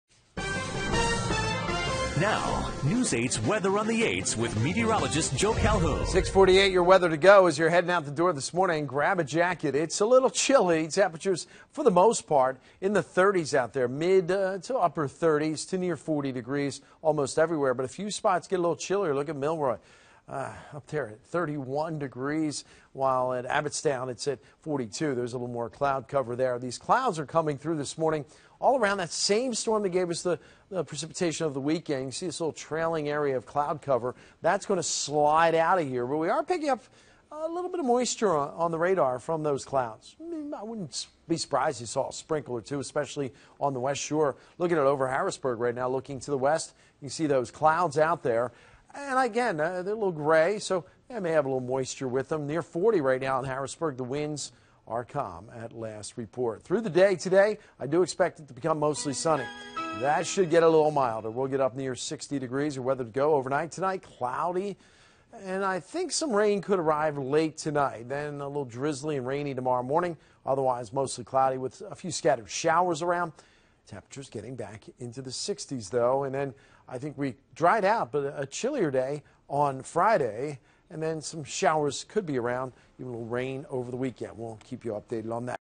Expect mostly sunny, milder conditions today
Watch Joe Calhoun's forecast.